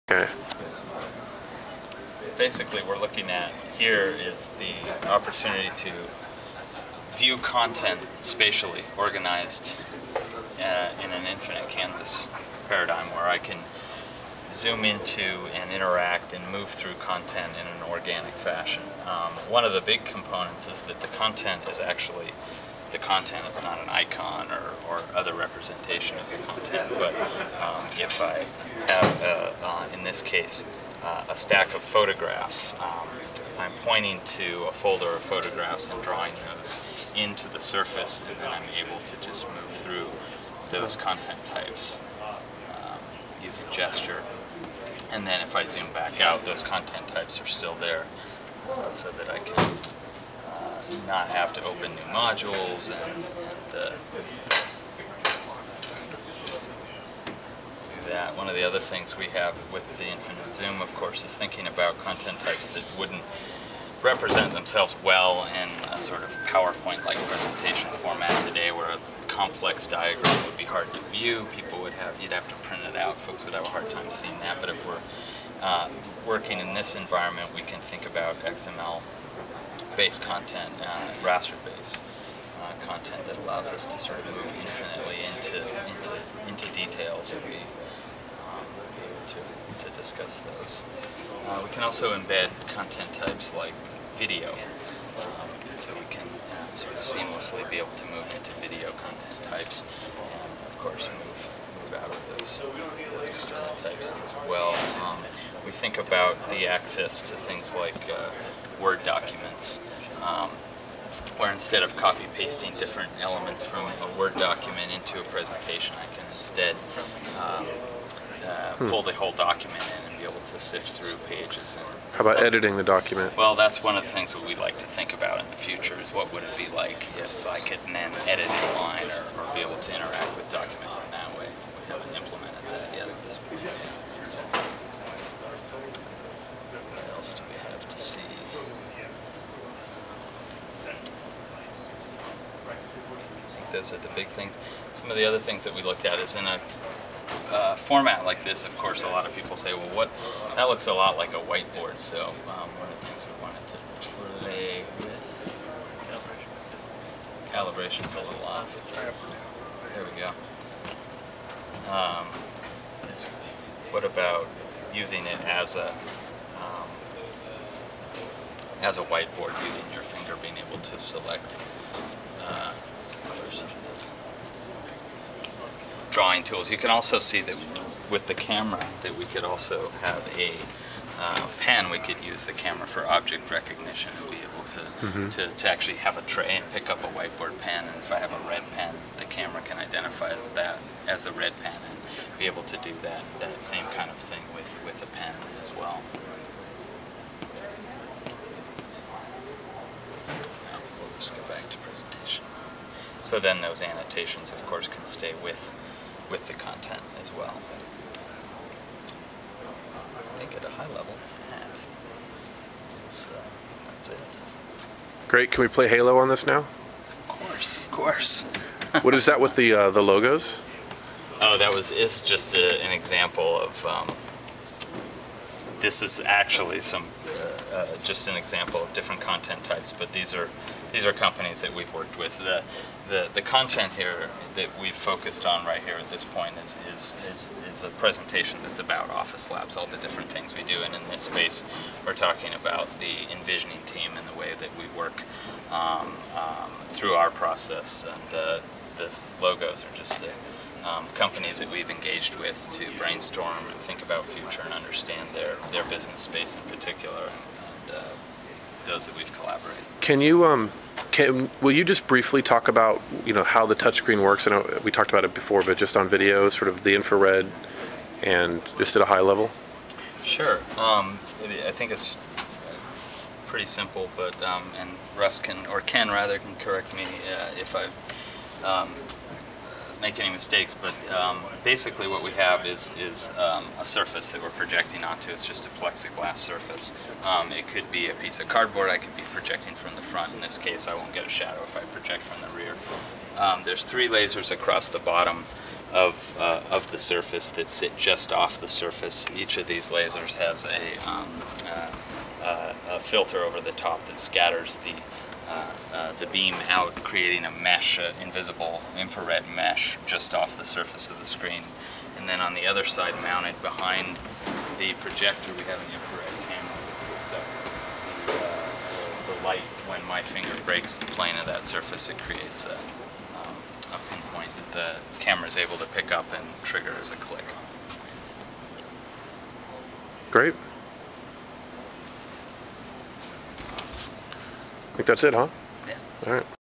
TechCrunch's Demo of Microsoft TouchWall
Demo by Ian Sands of Microsoft of TouchWall on May 13, 2008